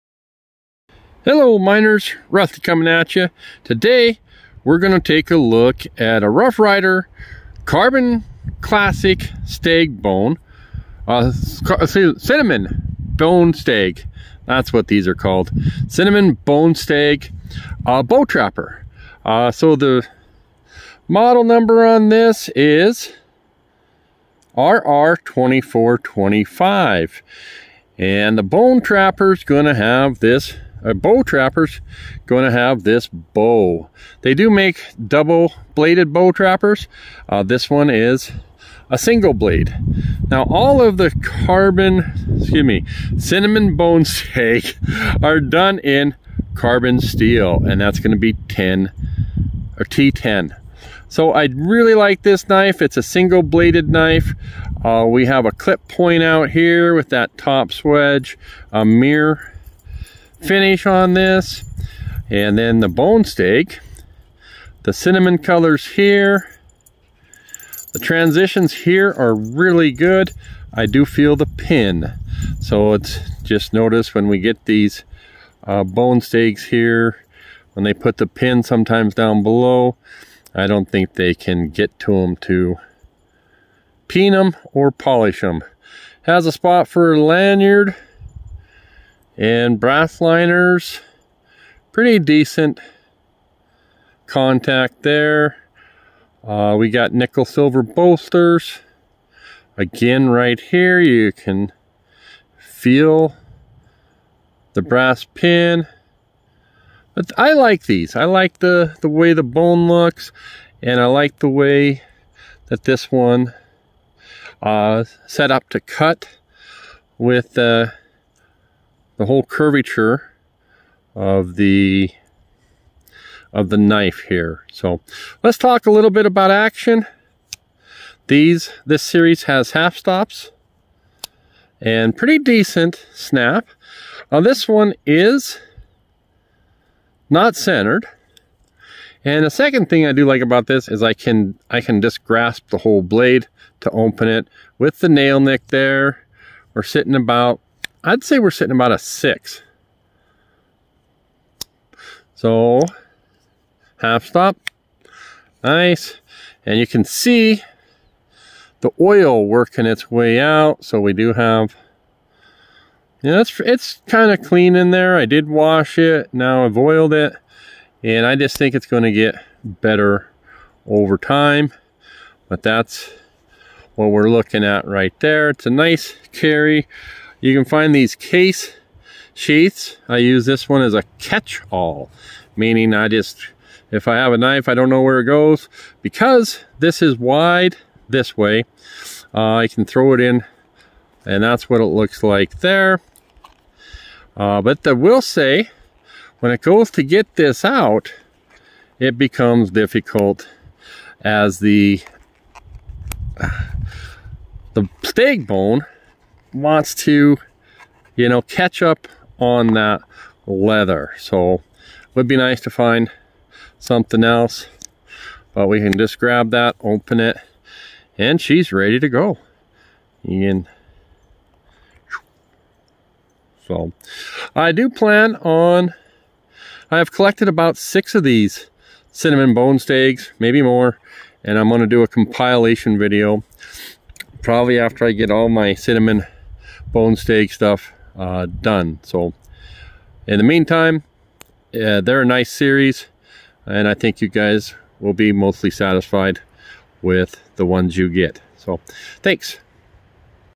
ROUGH RYDER CLASSIC CINNAMON BONE STAG BOW TRAPPER RR2425 ~ Check out the curves on this one!
OVERVIEW
All the knives in the Rough Ryder Classic Cinnamon Bone Stag Series feature mirror polished carbon steel blades with match strike pulls. The handles are cinnamon bone stag with brass and black synthetic liners, nickel silver pins, and nickel silver bolsters. This Rough Ryder Classic Cinnamon Bone Stag Bow Trapper has a clip point blade.

Remember, Rough Ryder is always built by hand and backed for life.

SPECIFICATIONS
Specifications
Brand Rough Ryder
Country of Manufacture China
Finish Mirror Polish
Edge Type Plain
Frame / Liner Brass
Knife Type Pocket Knives
Opener Match Strike Pulls
Handedness Ambidextrous
Country Of Origin China
Lock Type Slip Joint
Best Use Everyday Carry
Product Type Knife
Model # RR2425
Knife Style Trapper
Blade Style Clip Point
Handle Color Stag
Handle Material Bone Stag
# Of Blades 1
Blade Material Carbon Steel